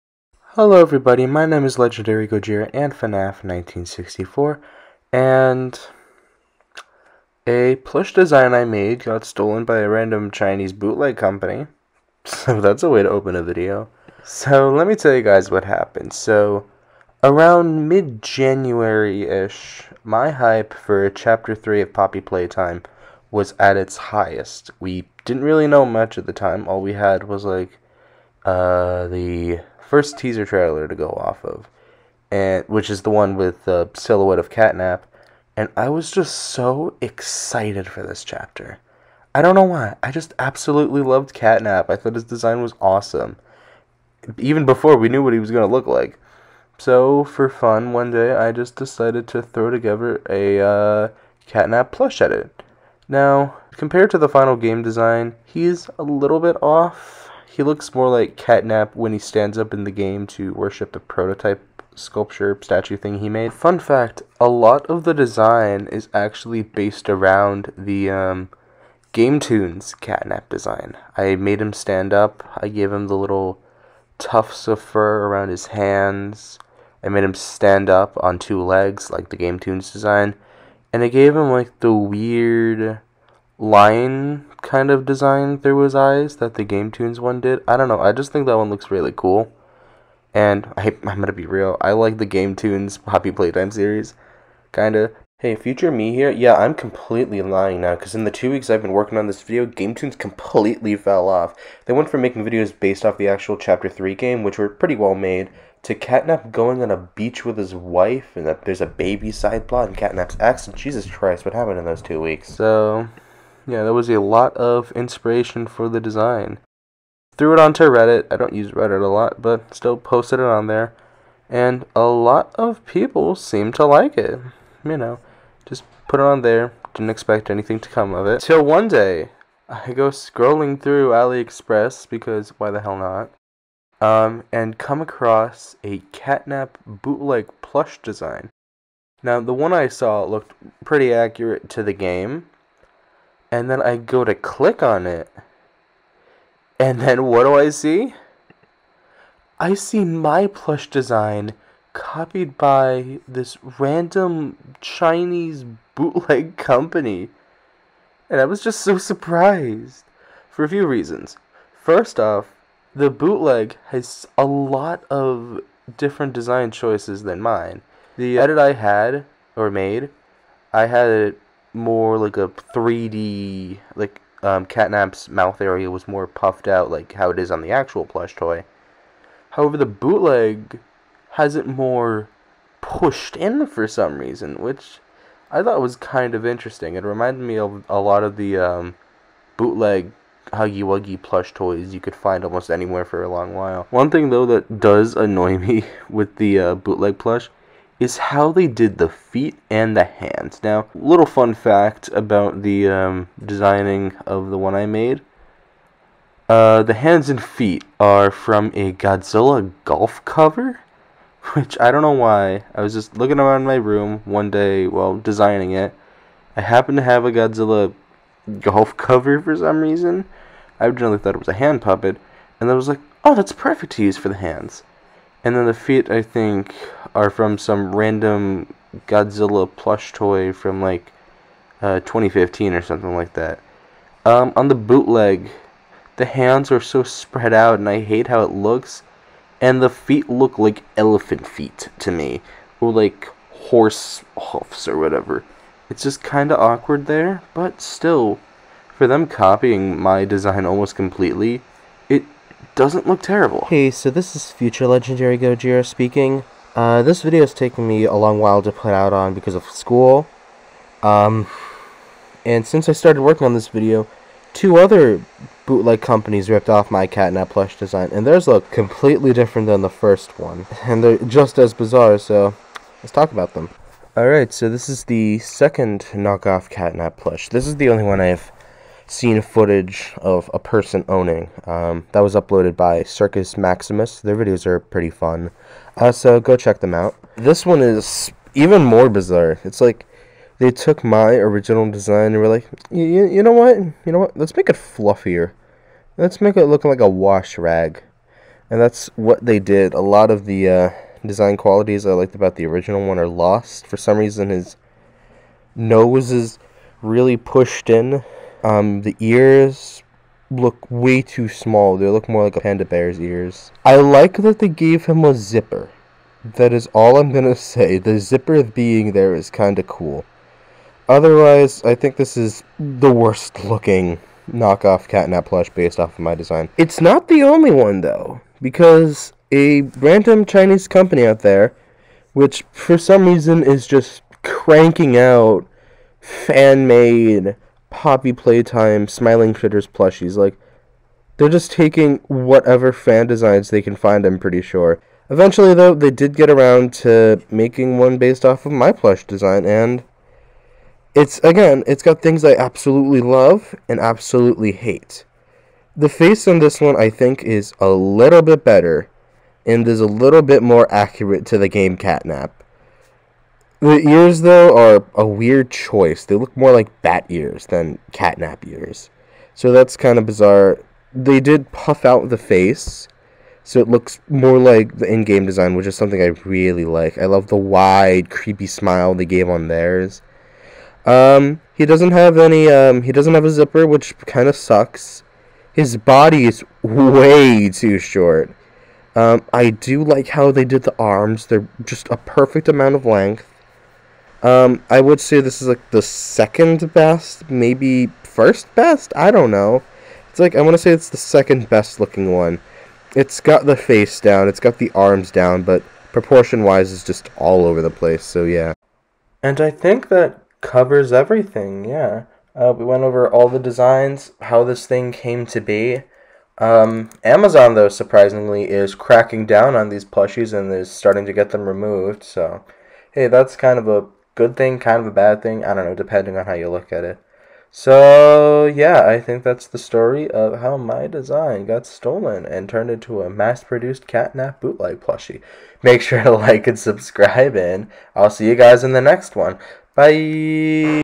MY CATNAP PLUSH GOT MADE INTO A BOOTLEG!!!
In this video I all the story about how my Catnap plush edit got STOLEN and turned into a bootleg plush toy. this video took a long time to make due to school and personal stuff but I'm glad I'm finally able to show it to you all!

important links